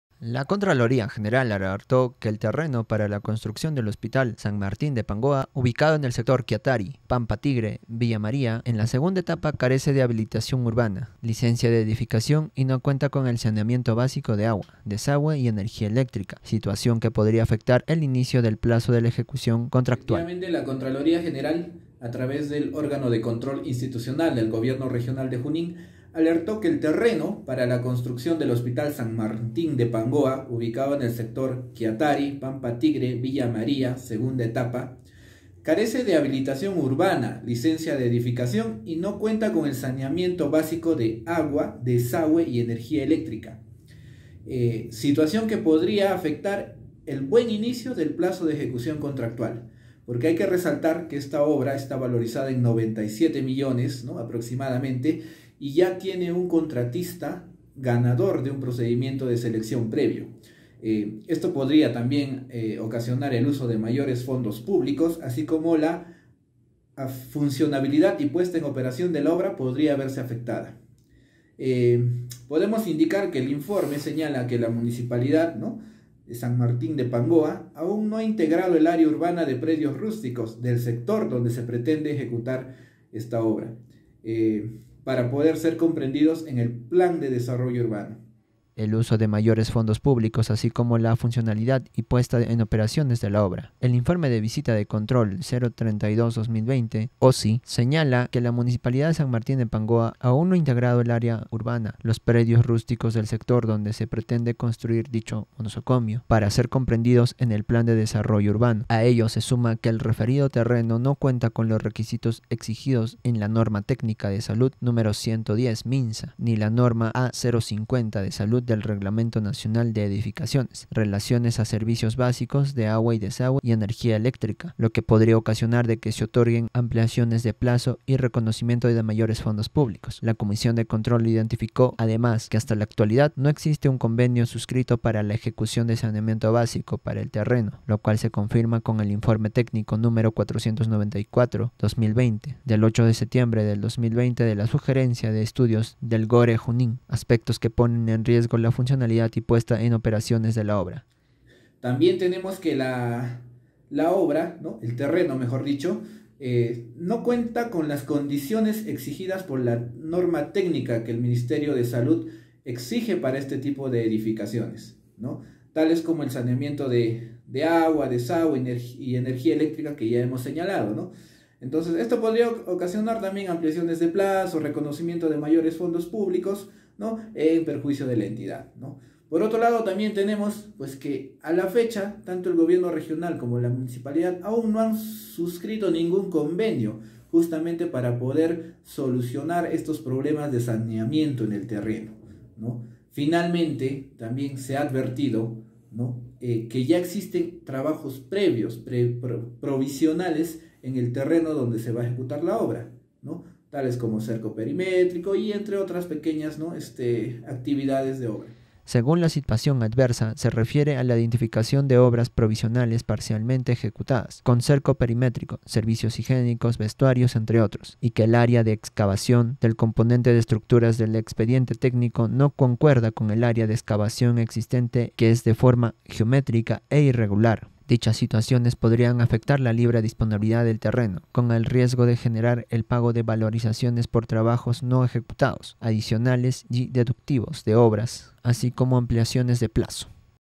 PANGOA: Terreno para habilitación de nuevo hospital carece de habilitación urbana | KDNA NOTICIAS
La Contraloría General alertó que el terreno para la construcción del Hospital “San Martín de Pangoa”, ubicado en el sector Kiatari – Pampa Tigre- Villa María - II Etapa, carece de habilitación urbana, licencia de edificación y no cuenta con el saneamiento básico de agua, desagüe y energía eléctrica, situación que podría afectar el inicio del plazo de la ejecución contractual, el uso de mayores fondos públicos, así como, la funcionabilidad y puesta en operaciones de la obra.